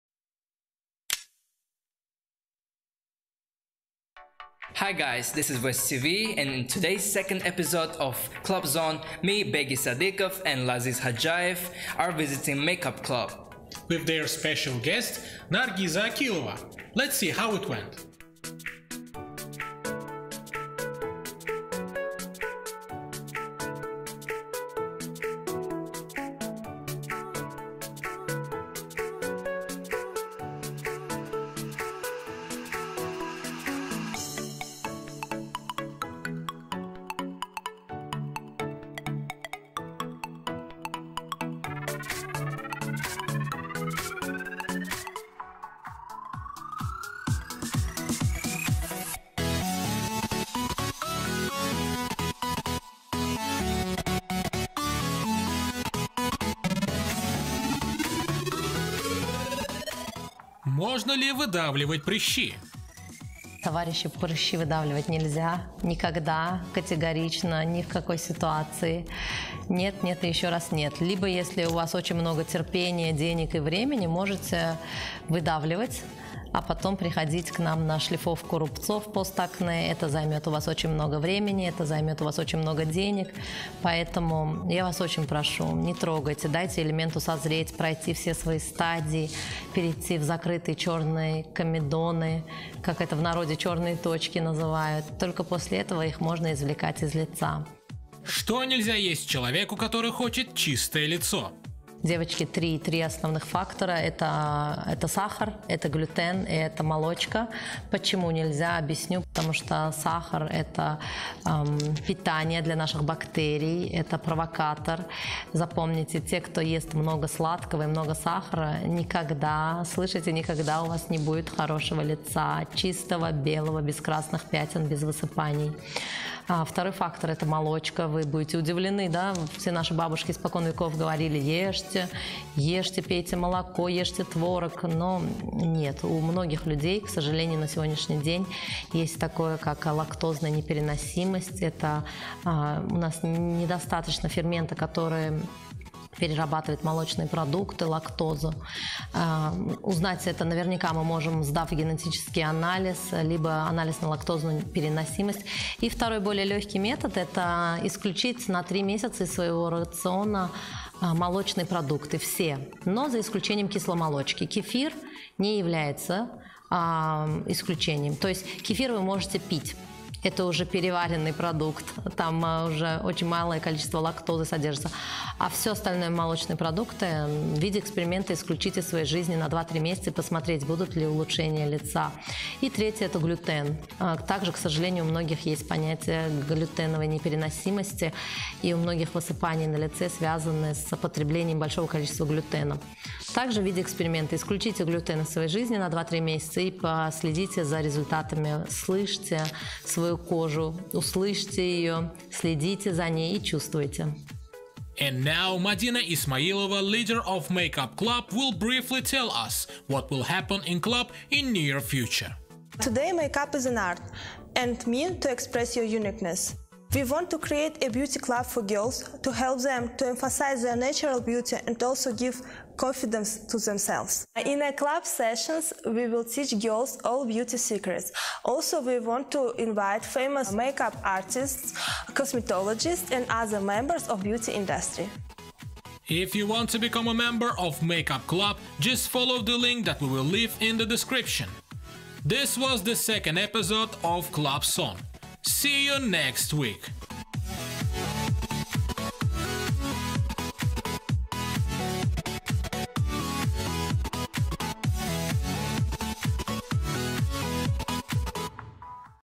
Clubs ON! | Make Up Club
In second episode of Clubs ON! Laziz Khodjaev and Begi Sodikov visited guest lecture of cosmetologist Nargiza Akilova and met leader of Make Up Club Madina Ismailova.